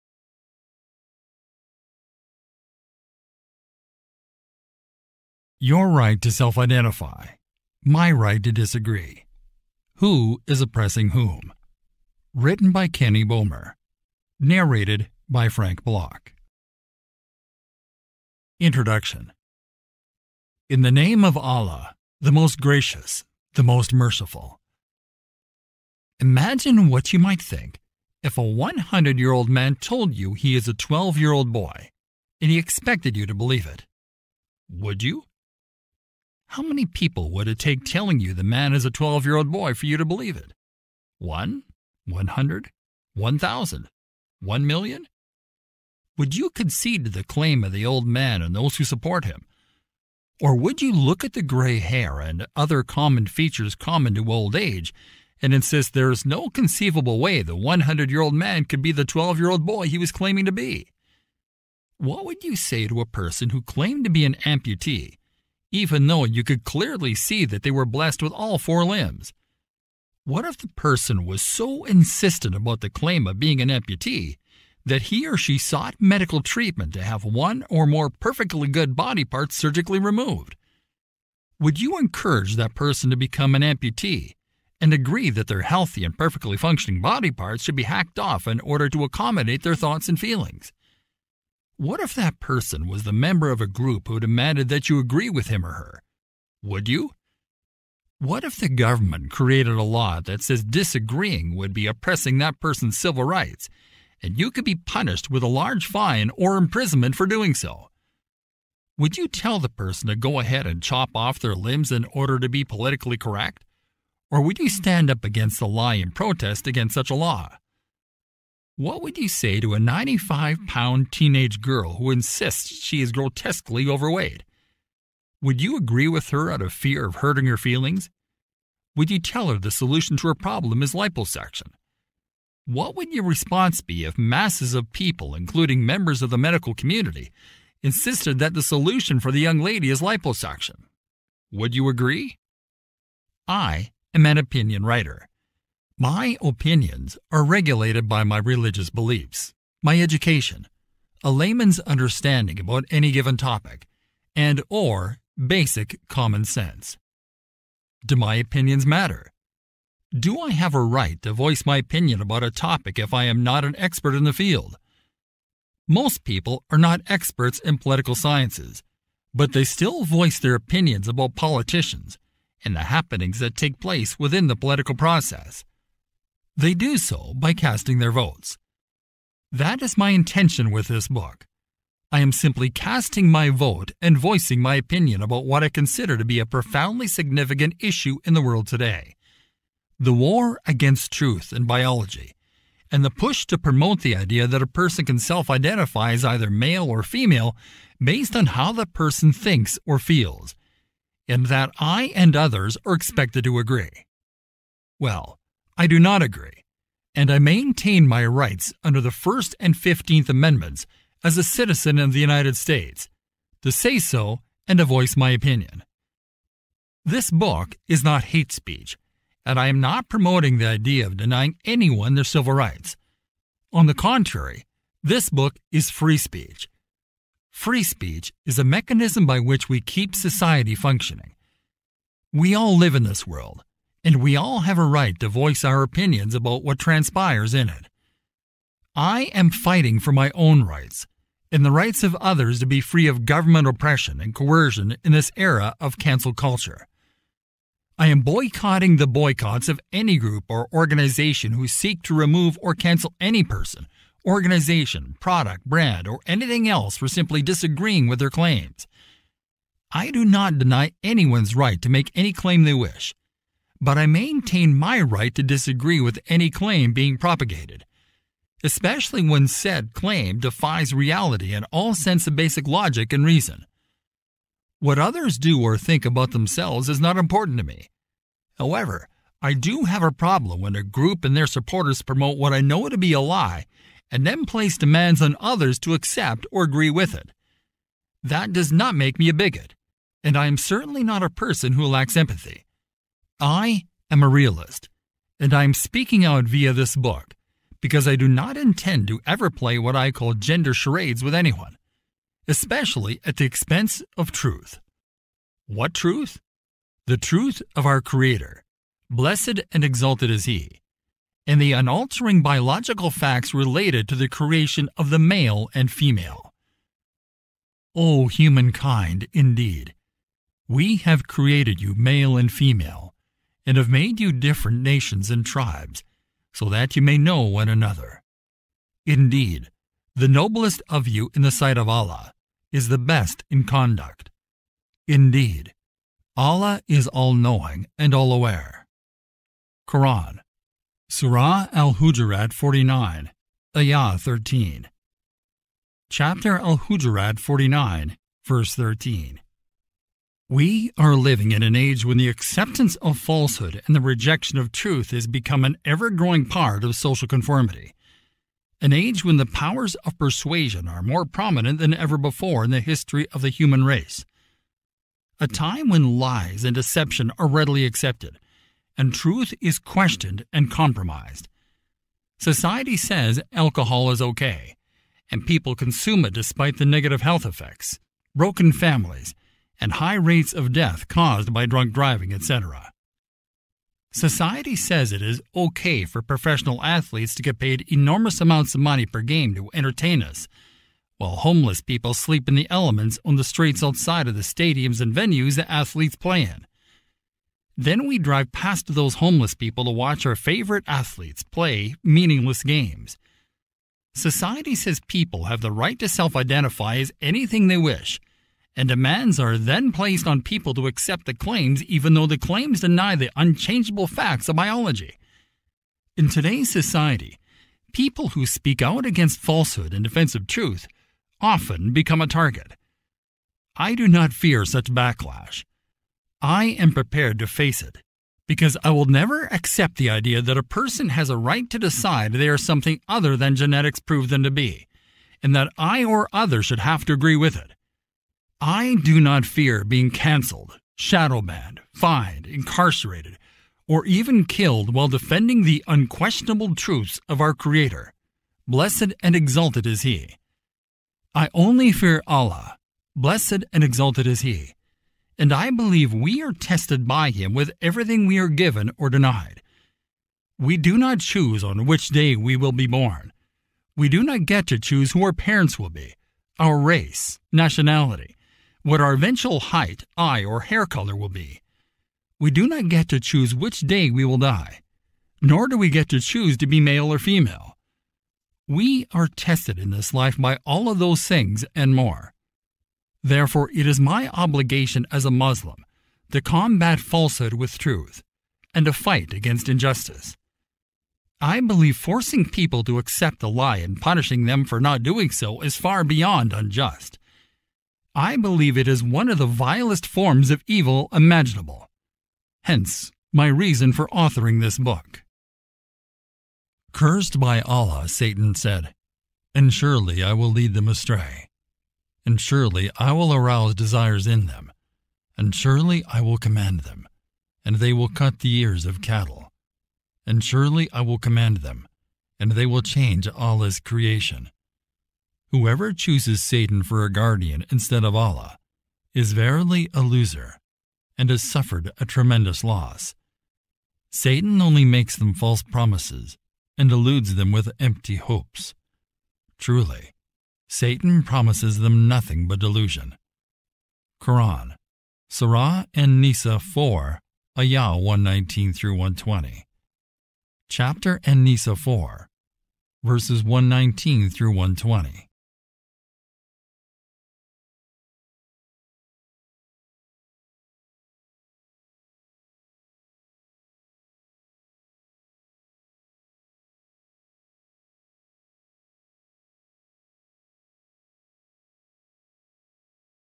Your Right to Self-Identify, My Right to Disagree Audiobook Series
This is the Audiobook Series (Introduction) from the book, Your Right to Self-Identify, My Right to Disagree, written by Author Kenny Bomer.  Narrated by Frank Block